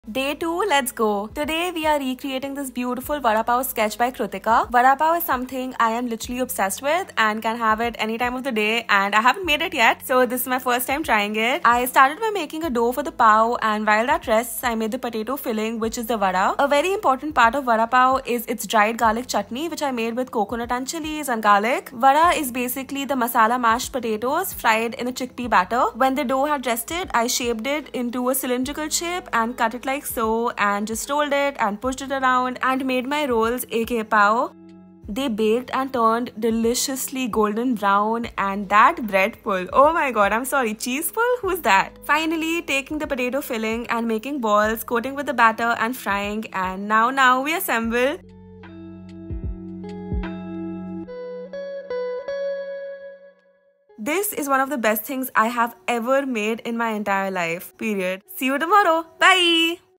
vada pav: day 2 of you draw i eat
pav (dinner rolls):
- 2 cups flour
- 1 tbsp honey
- 1 1/2 tsp yeast
- 1/2 cup milk
- 1/2 cup water
- 1 tsp salt
- 2 tbsp butter

mix, knead and rest for 2 hrs in an oiled utensil

shape into balls and rest again for 1 hr before baking at 190°C for 25 min

potato filling:
- 3 boiled potatoes
- 1 tbsp oil
- 1/2 tsp mustard seeds
- 1/2 tsp turmeric
- 1/2 tsp chilli powder
- curry leaves
- 2 green chilli
- 1/2 tsp cumin

dry garlic chutney:
- 1/4 cup shredded coconut
- 5 dry red chilies
- lots of garlic
- 1/4 tsp hing

chickpea flour batter:
- 1 cup chickpea flour
- water
- 1 tsp turmeric
- 1 tsp chilli powder
- 1 tsp salt